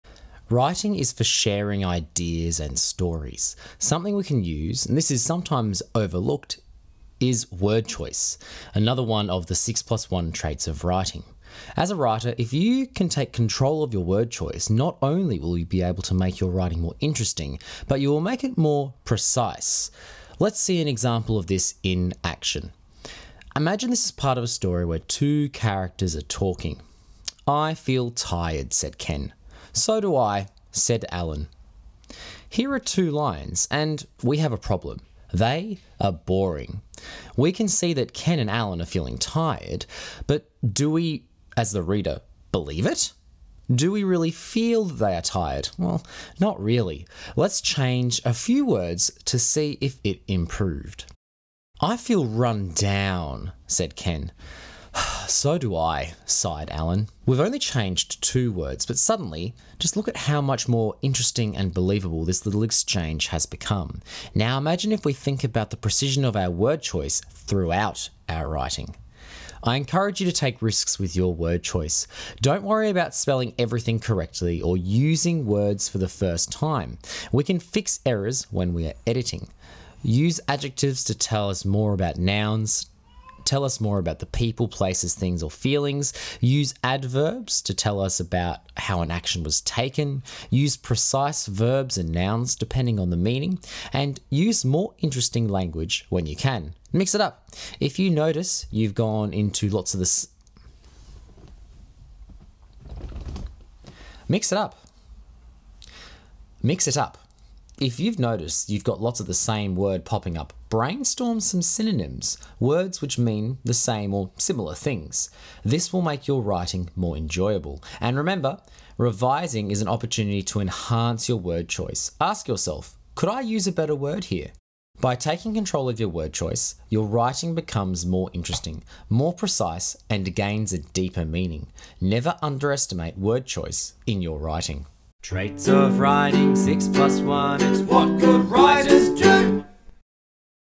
Word Choice | 6+1 Traits of Writing
In this video, we look at the 6+1 Traits of Writing trait of Word Choice. The words you choose can make your writing brilliant or boring. Choosing the right words makes your writing more interesting and precise.

We investigate some examples of Word Choice and how it can elevate our writing.